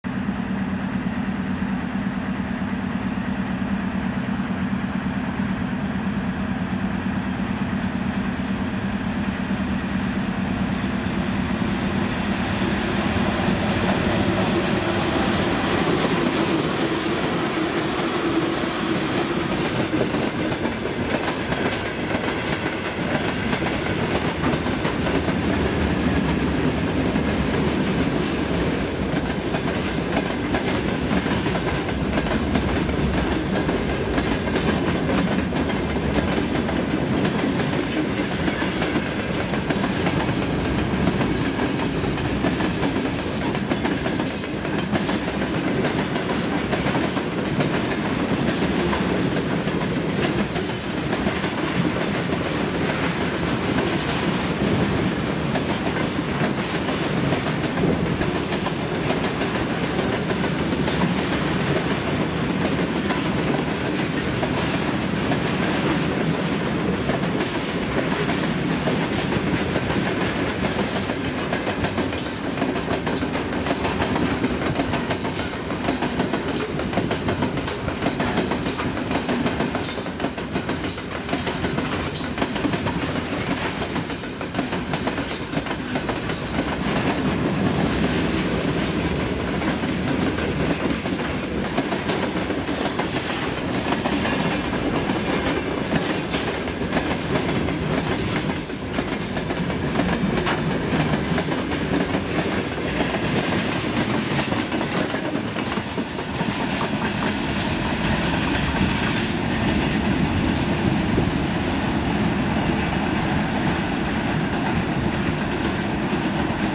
Электровоз ВЛ11 с грузовым поездом, ст. Яганово, 20 сентября 2023 г.
Электровоз ВЛ11 следует по ст. Яганово.
20 сентября 2023 г.